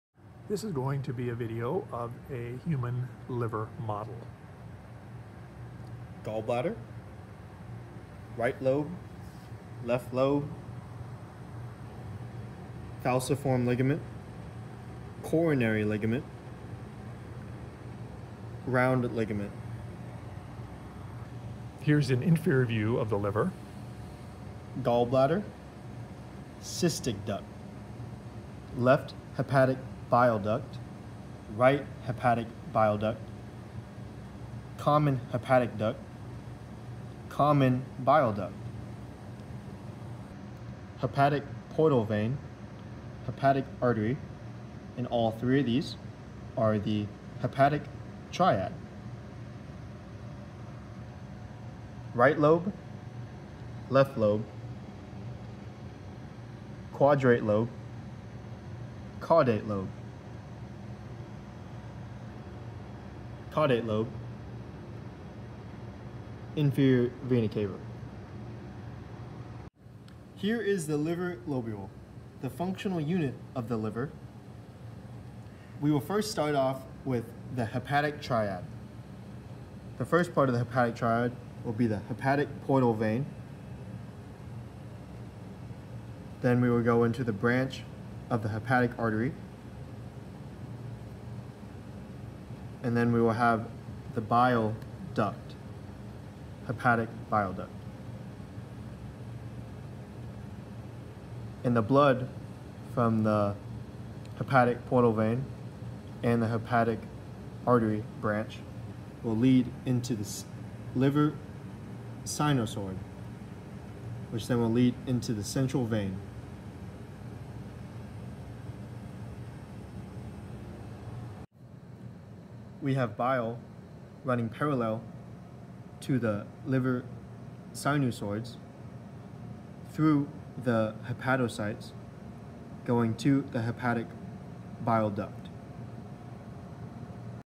Liver gross and microscopic Anatomy
The gross anatomy of the liver and the microscopic anatomy of a liver lobule are reviewed on plastic human models